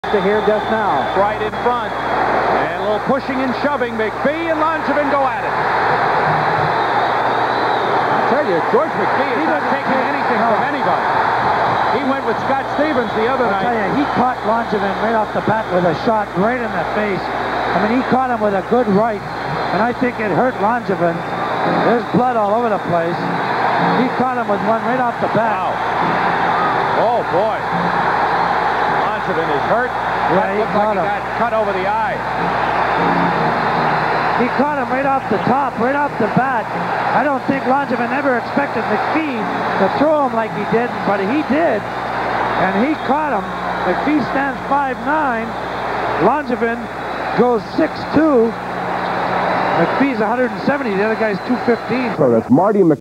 George McPhee pummels Dave Langevin 11-9-84
George McPhee of of the Rangers bloodies Dave Langevin of the Isles.